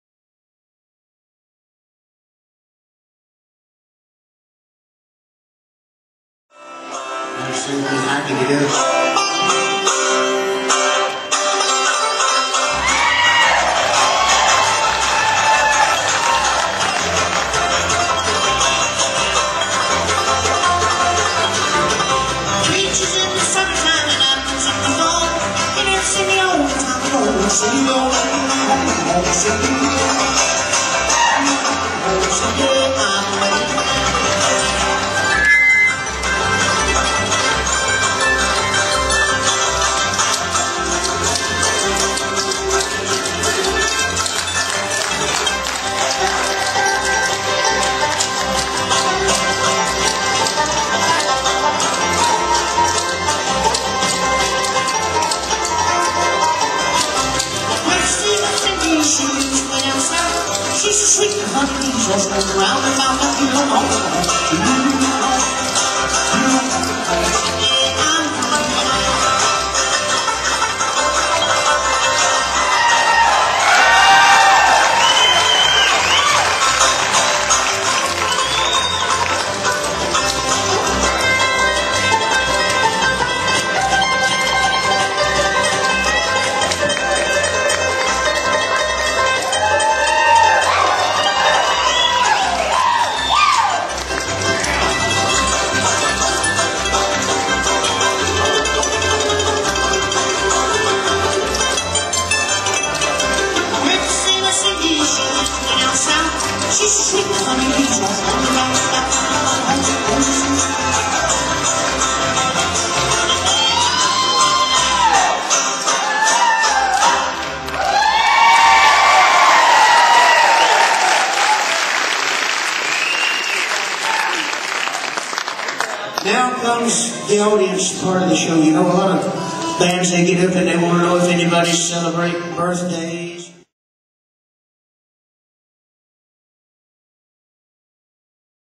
Big Country Bluegrass-Clawhammer Banjo
Big Country Bluegrass thrills the house in Princeton, WV with a clawhammer banjo song.